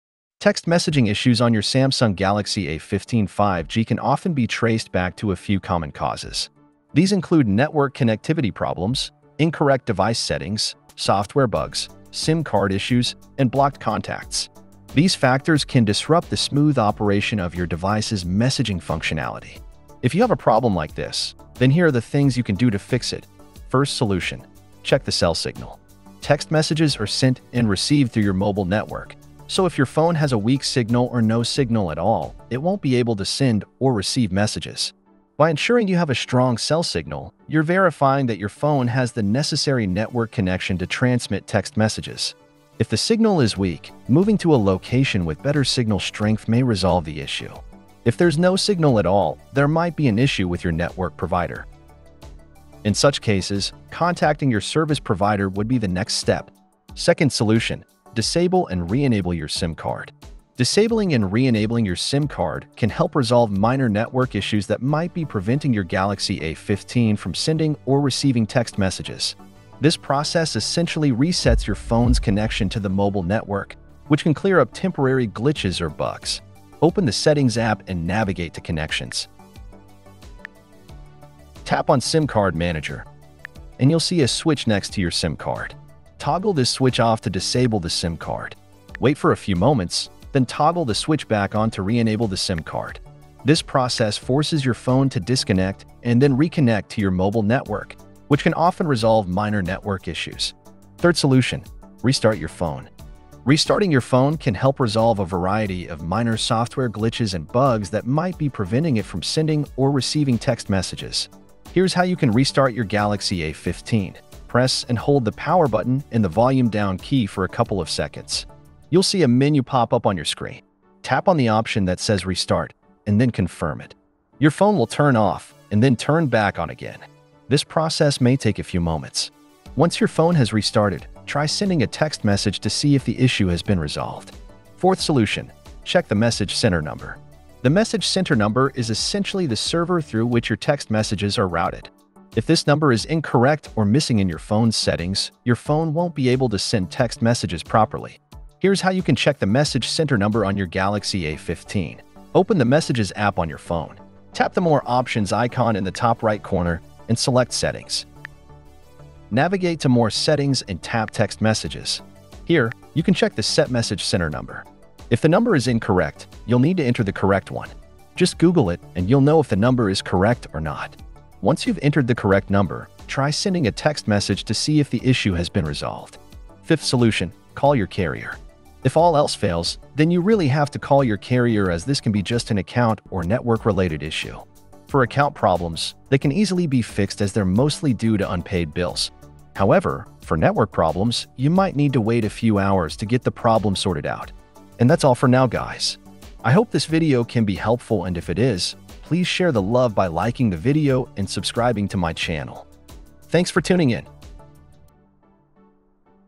[SOLVED] Galaxy A15 5G Can’t Send and Receive Text Messages
Text messaging issues on your Samsung Galaxy A15 5G can often be traced back to a few common causes. These include network connectivity problems, incorrect device settings, software bugs, SIM card issues, and blocked contacts. These factors can disrupt the smooth operation of your device’s messaging functionality.

0:00 Intro
0:25 First solution: Check the cell signal
0:59 Second solution: Disable and re-enable your SIM card
1:41 Third solution: Restart your phone
2:15 Fourth solution: Check the Message Center Number
2:57 Fifth solution: Call your Carrier
3:19 Subscribe